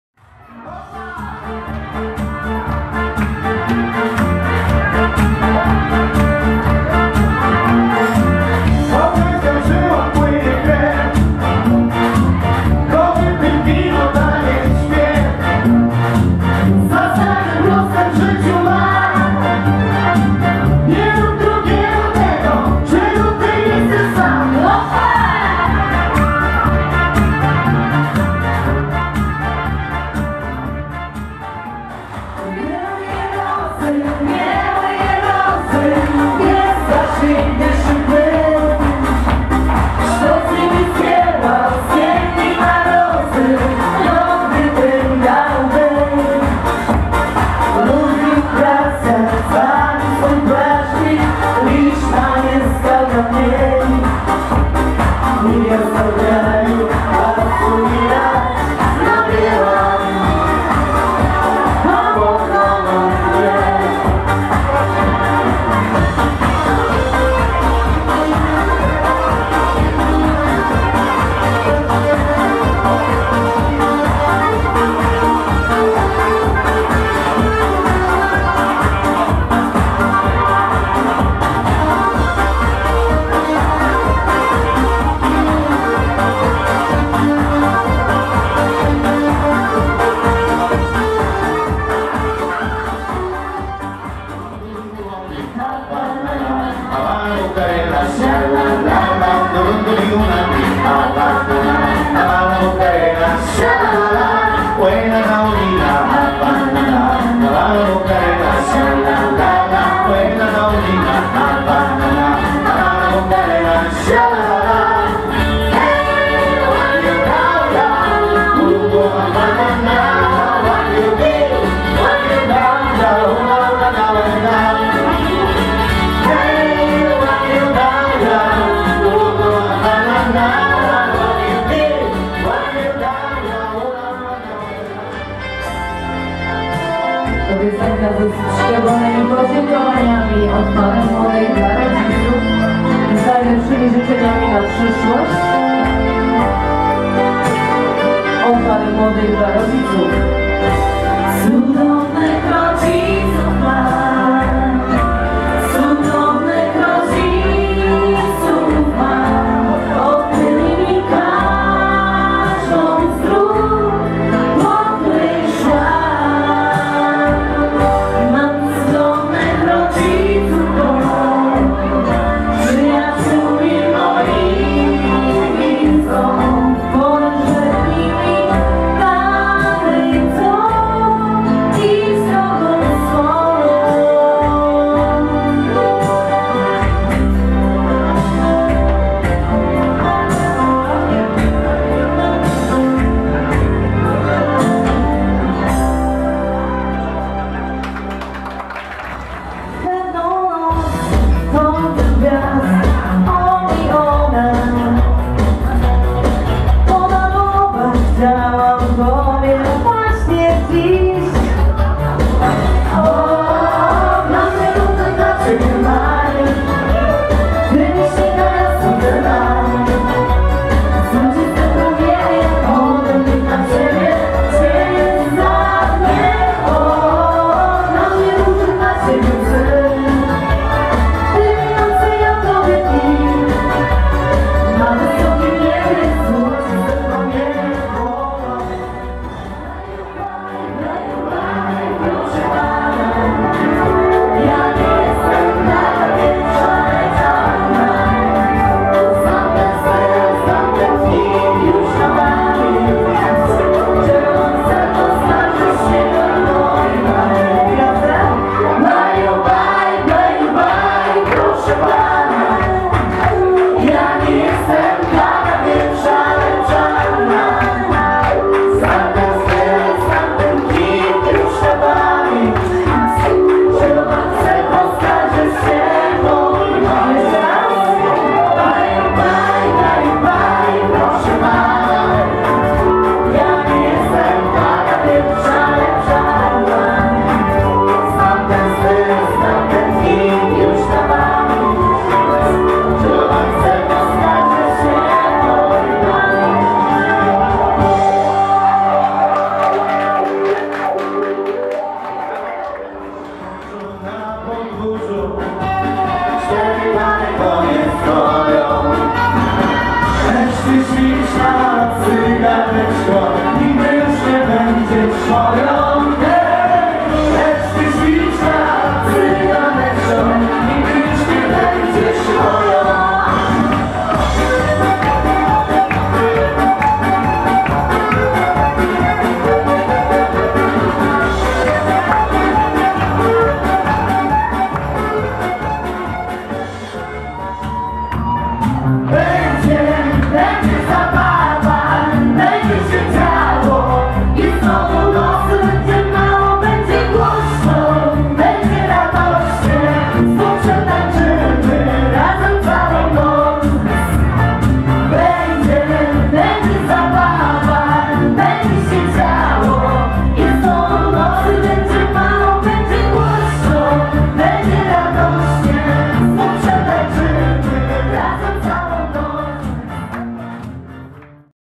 ExProblem wesele Konstancin-Jeziorna 30.08.2014
ExProblem covery przyjęcie weselne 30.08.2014 Centrum Konferencyjno-Hotelowe "Alicja" Konstancin-Jeziorna.